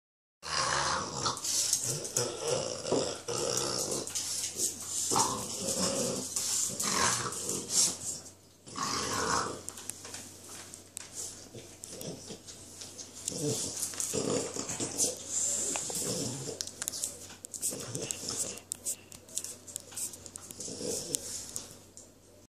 Le combat des chien
Lisy et éragonne font un combat